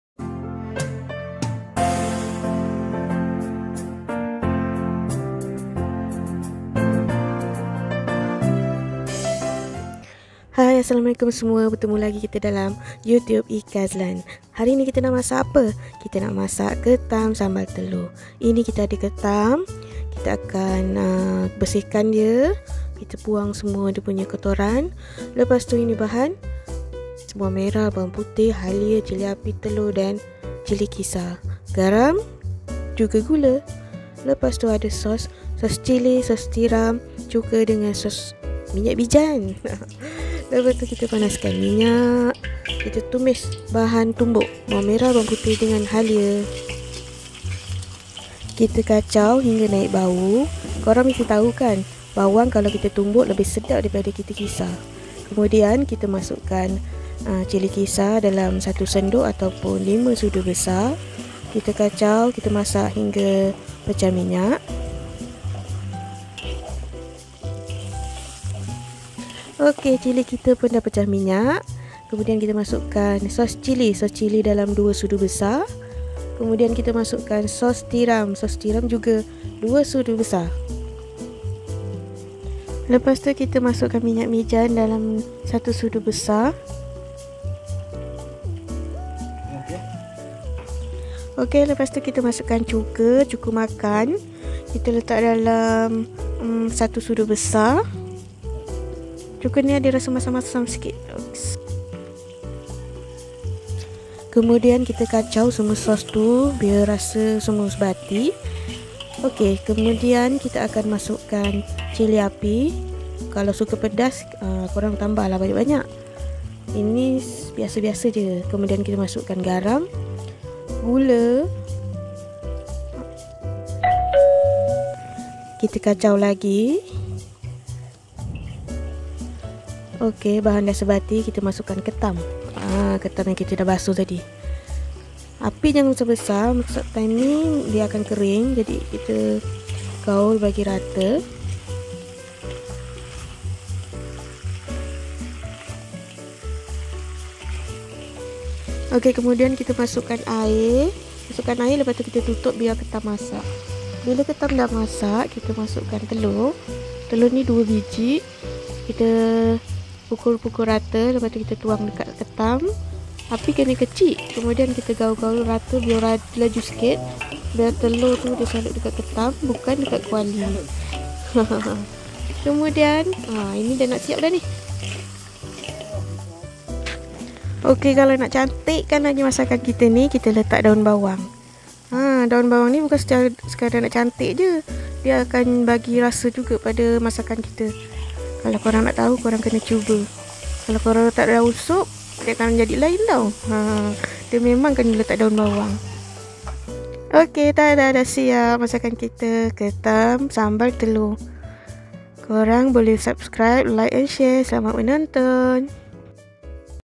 (RESIPI) CARA MASAK KETAM SAMBAL TELUR
BAHAN-BAHAN

KETAM
2 BIJI TELUR
1 SENDUK CILI KISAR / 5 SUDU BESAR
2 SUDU SOS TIRAM
2 SUDU SOS CILI
1 SUDU BESAR MINYAK BIJAN
1 SUDU BESAR CUKA MAKAN
DAUN BAWANG
CILI API
GARAM
GULA

TUMBUK
3 ULAS BAWANG MERAH
3 ULAS BAWANG PUTIH
1 INCI HALIA